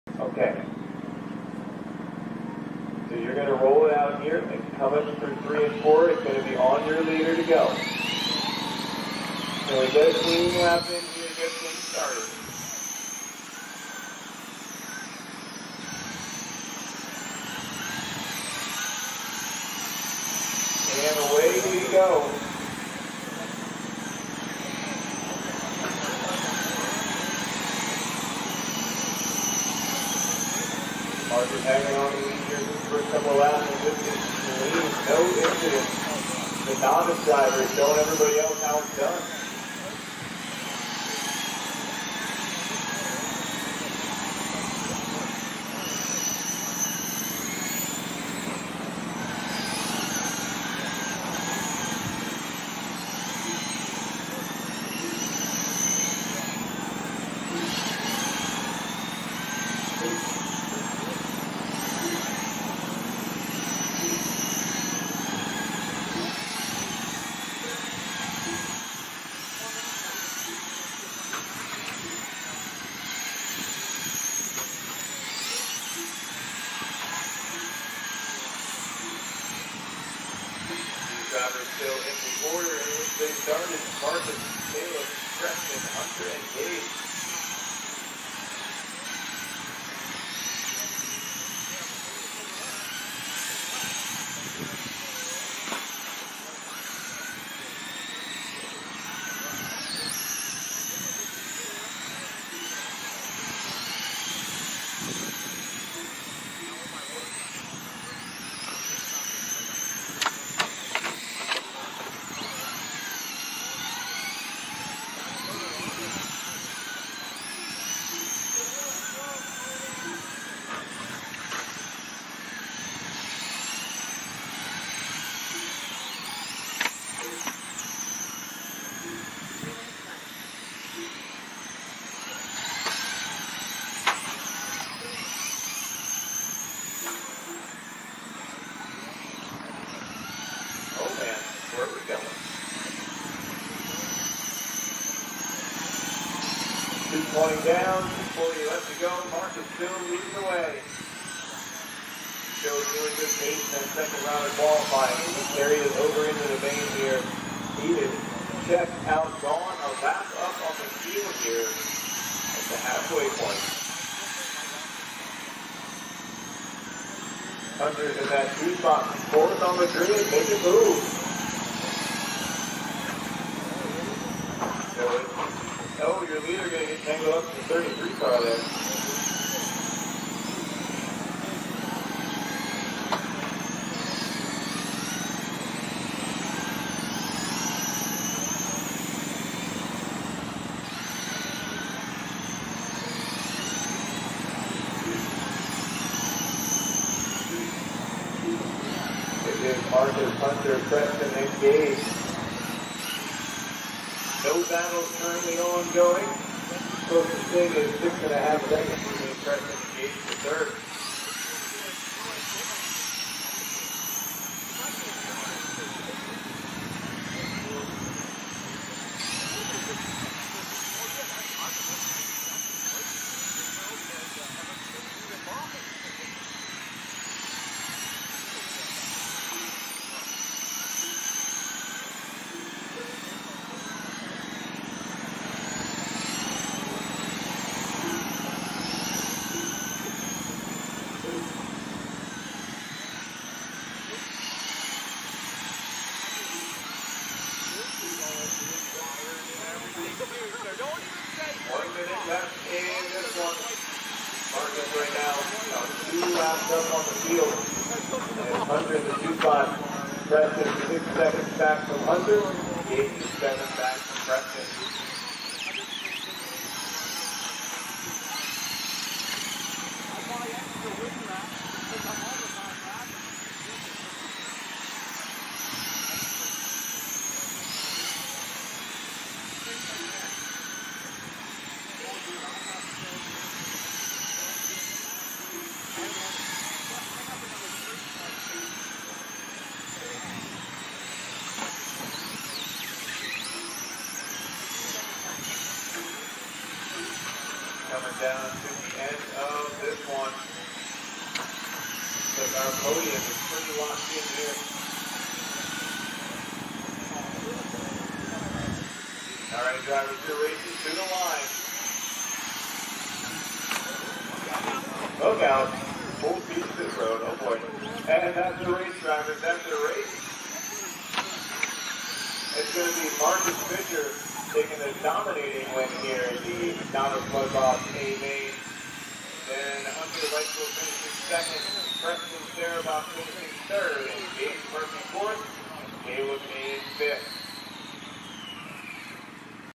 LCRC 2022 Oval Season Opener- Race 2: Novice Mudboss (Novice Mudboss A-Main)
Race 2: Novice Mudboss (Novice Mudboss A-Main)
1 Marcus Fisher
2 Hunter Leitzel
3 Preston Sarabok
4 Gage Murphy
5 Caleb Sarabok